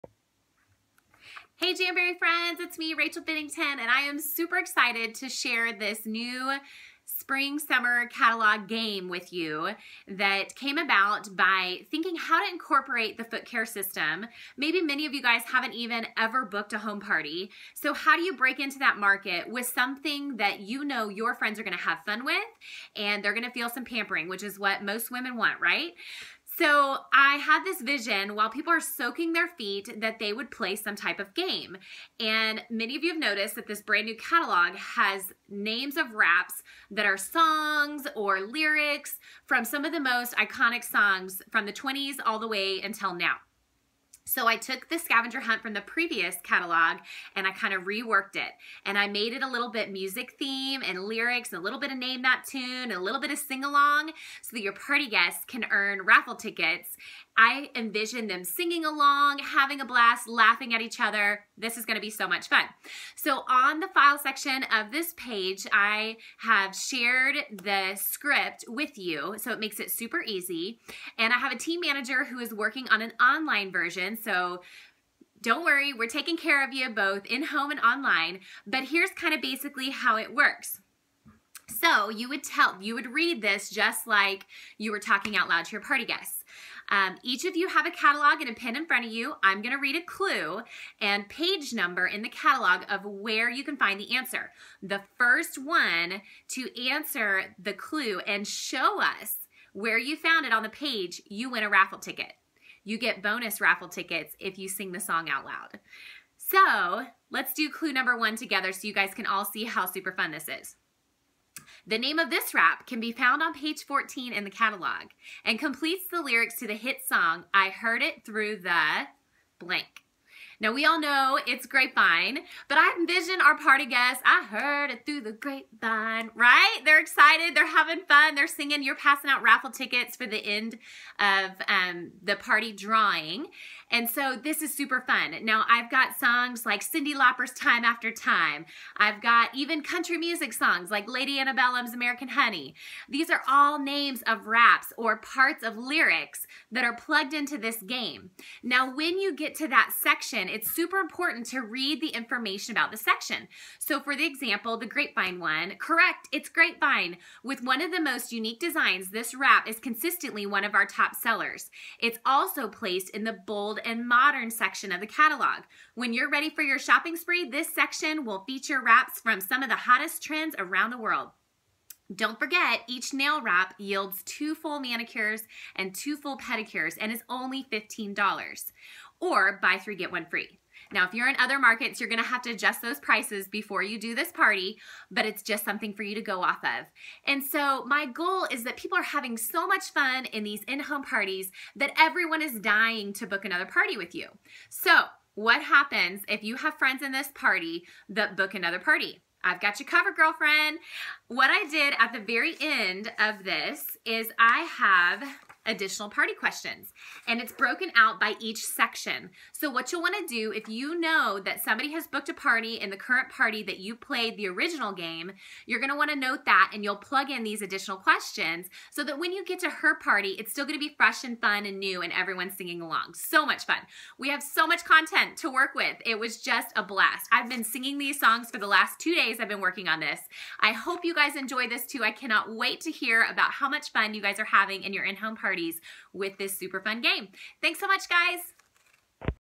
NEW In home party game your guests will JAM with! Jamberry Style
Amp up your FUN METER in your in home parties with this game! 2017 Spring/Summer Jamberry Catalog Scavenger hunt! Files found on our team pages:)